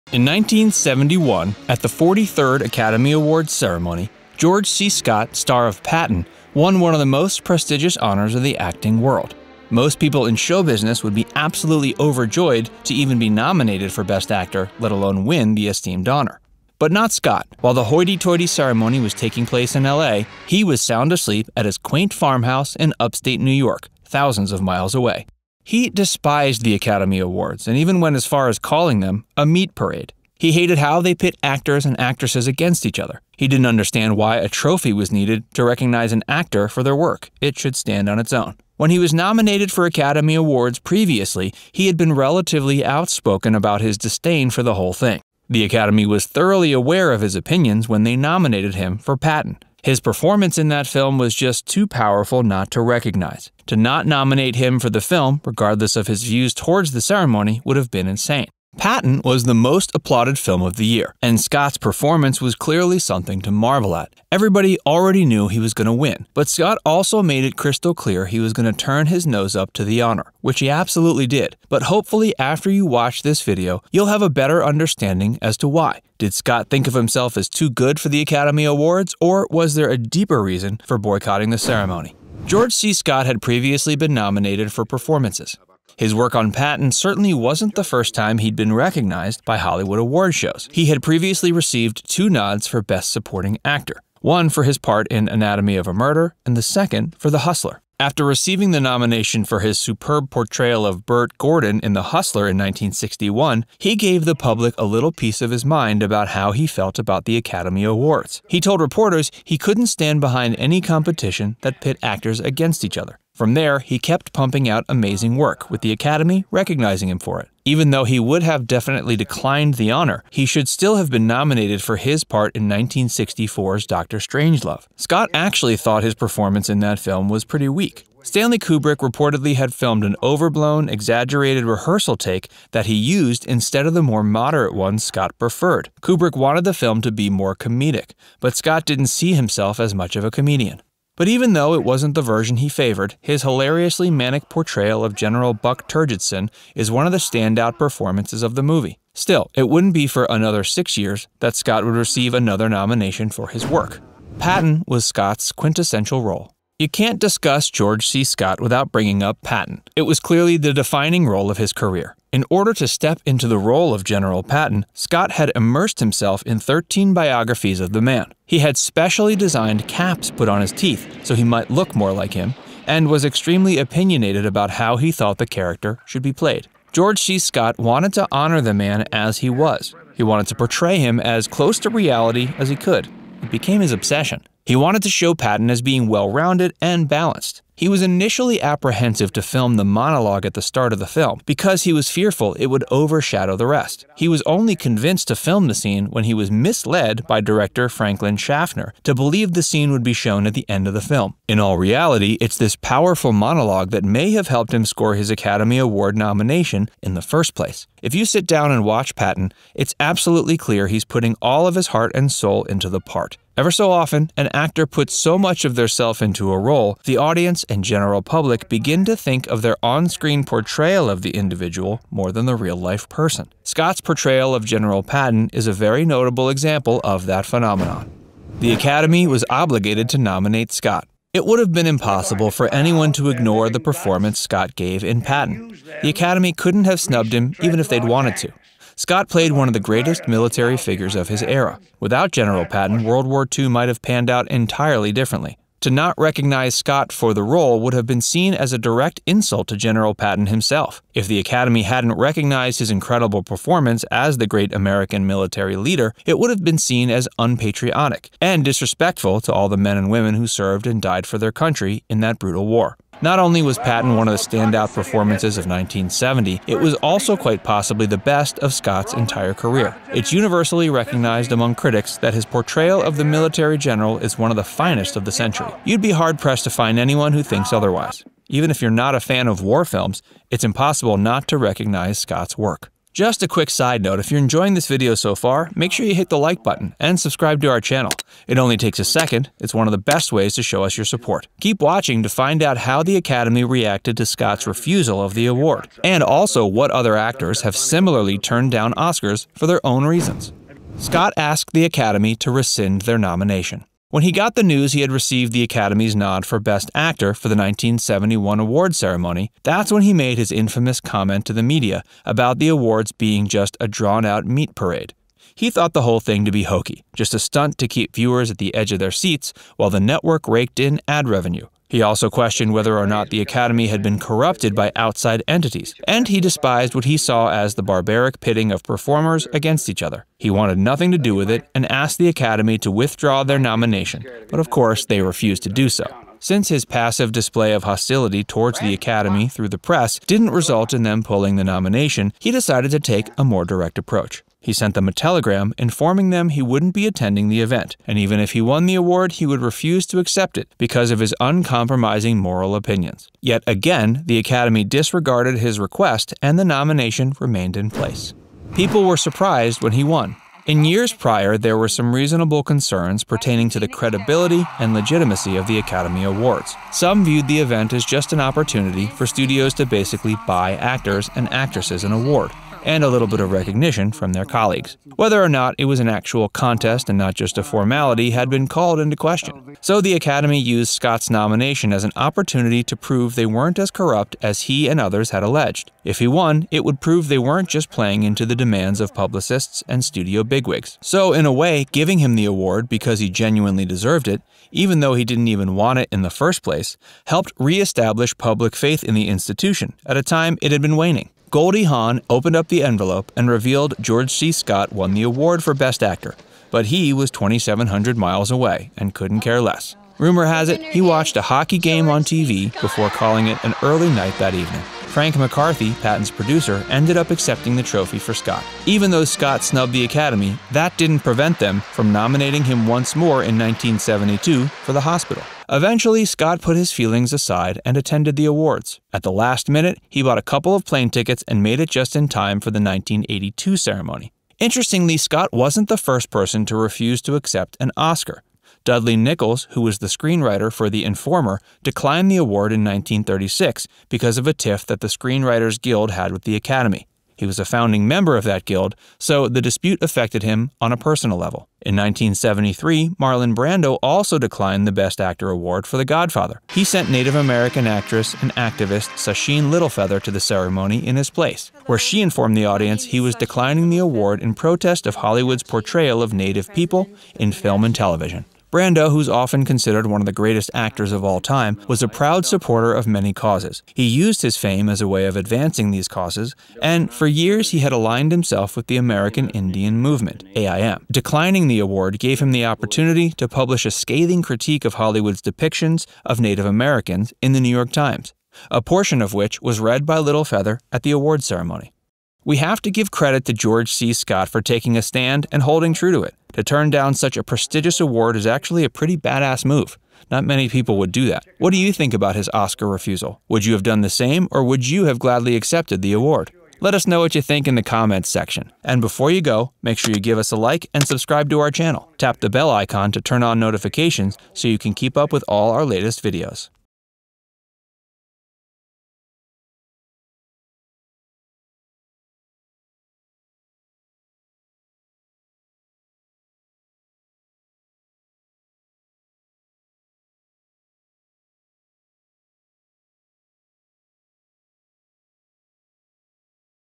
George C. Scott Refused an Oscar for Patton!
George C. Scott played World War II hero, General Patton, in the 1970 film Patton. When he was nominated for the Best Actor in a Film Oscar in 1971, however, he refused the honor. He didn't like how Award Show's such as the Academy Awards pitted actors against each other but he also had a lack of faith in the integrity of the Academy as well.

This video is going to show you why Scott refused the award, why the Academy nominated him in the first place, what the award symbolized at the time not only for Scott but also for the film industry, and also what other actors have refused Academy Awards as well.

George C. Scott is absolutely one of the finest actors of the 20th century. He doesn't need an award for that fact to be obvious. Some of his other noteworthy films include Stanley Kubrick's 1964 dark comedy Dr. Strangelove, and 1971s The Hospital. For the latter film, he was nominated yet again by the Academy for a Best Actor Award.

He would receive another Golden Globe Nomination for Best Actor in the 1978 double feature Movie Movie. He would also win a golden globe in 1997 for the mini-series 12 Angry Men. There is no question that he is an incredible actor, he just didn't need a trophy to tell him so.